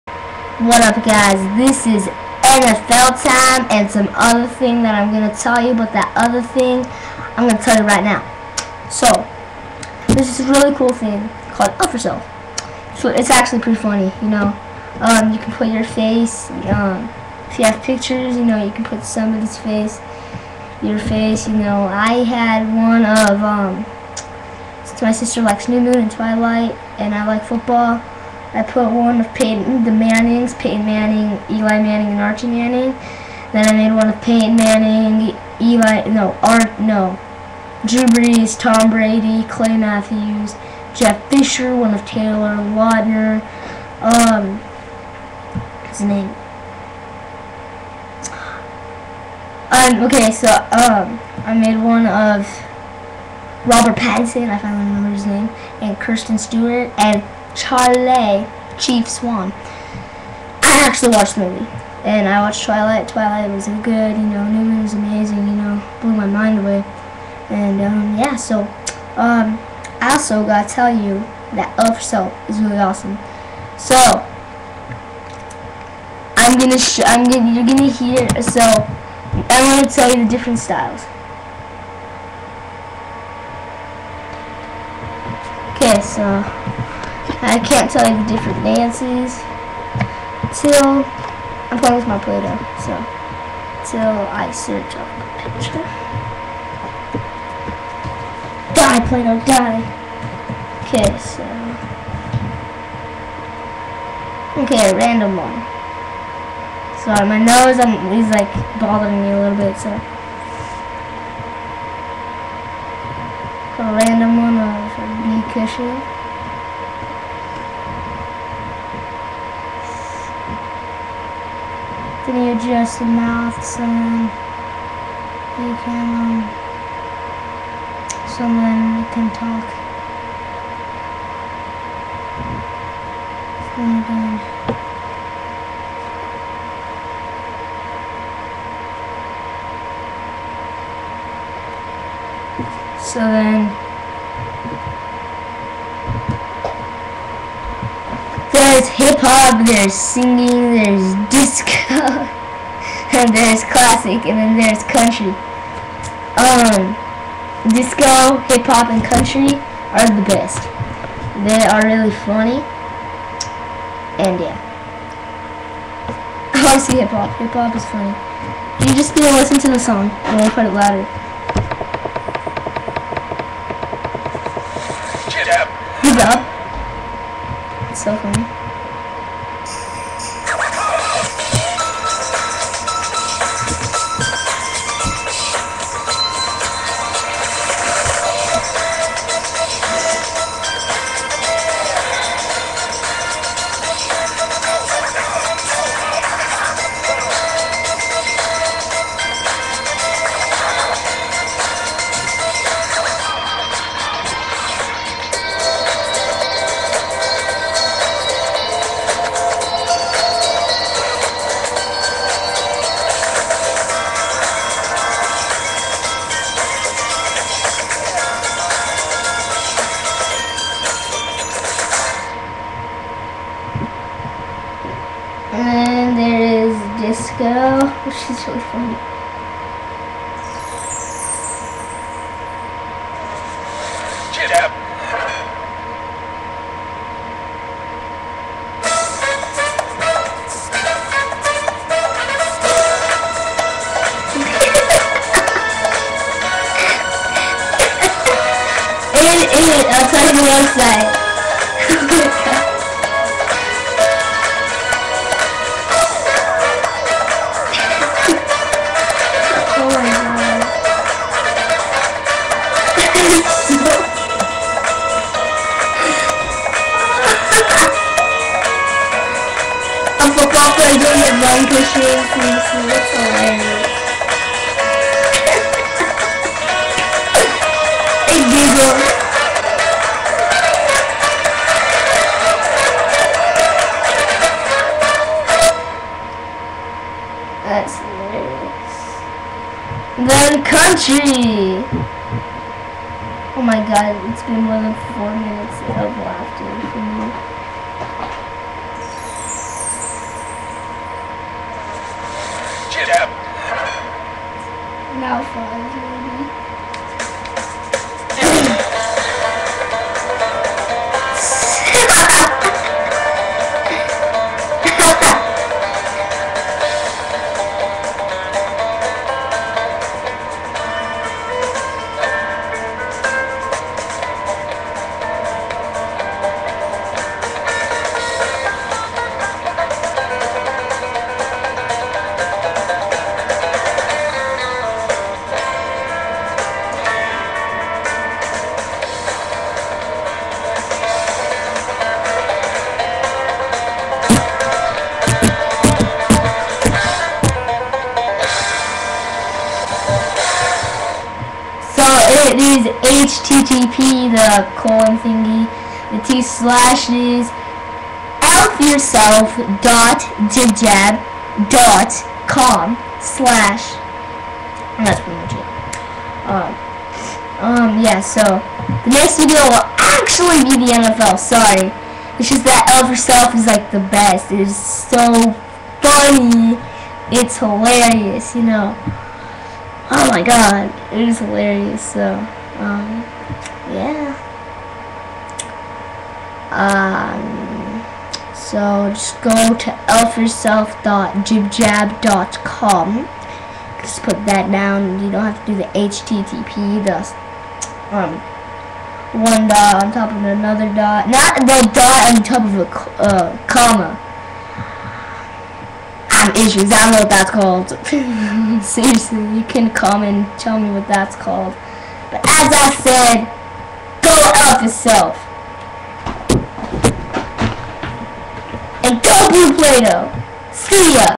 ELF YOURSELF
The website is, you can put your face on an elf and watch 5 dances, there are only five dances and you can only mak up until 5 elves and that it. the five dances are called, HIP HOP, SINGING, DISCO, CLASSIC, AND COUNTRY. These dances are hilarious and i reccomend you go and ELFYOURSELF. As i said, good night you little elves and MERRY CHRISTMAS.